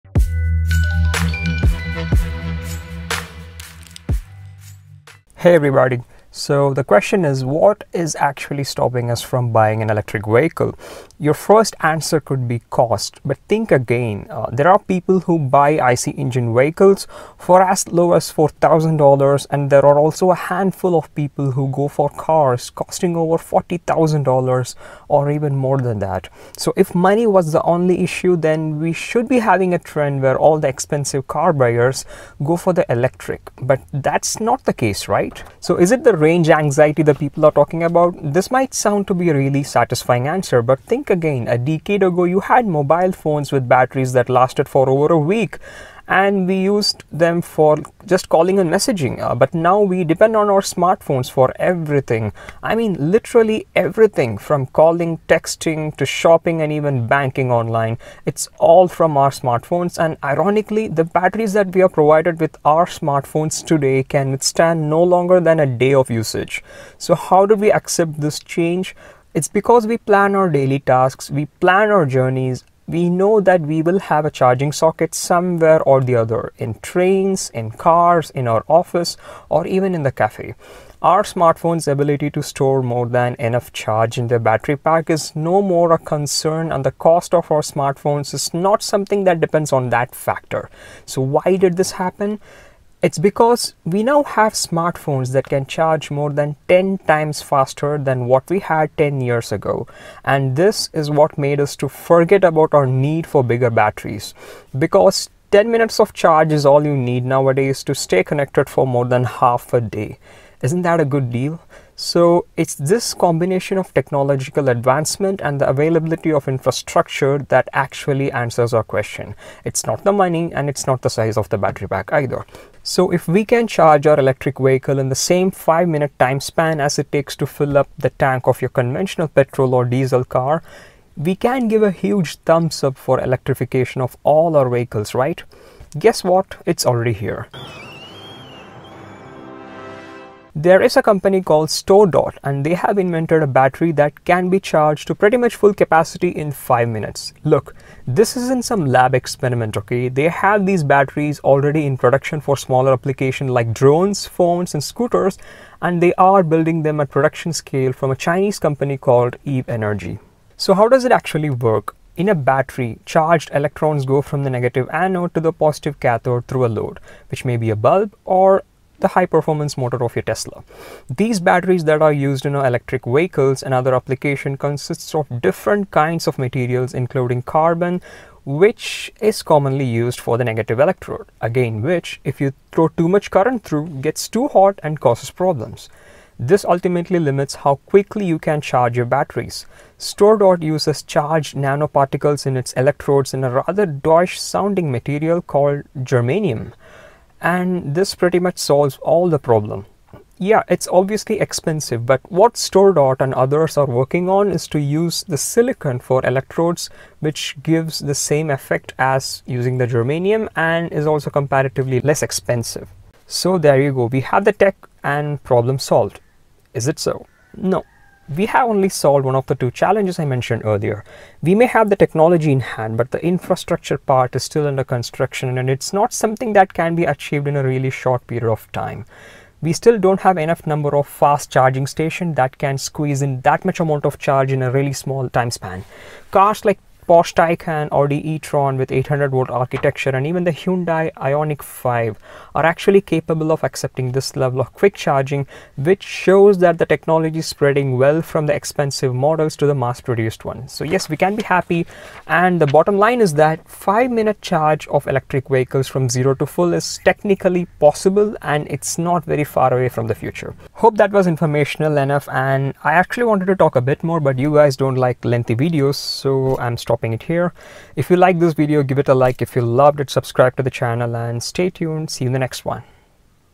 Why you don't need bigger batteries? | Economics of an EV
Why are we still not buying electric vehicles?. What is the real reason behind?. It is really surprising to know that it is neither the cost nor the range anxiety that is stopping us from shifting to electric vehicles, rather it is the availability of infrastructure that actually plays the key role.

Also, watch the video till the end to know how you can charge your electric vehicle in 5 minutes!. I'm not kidding, the technology is already here.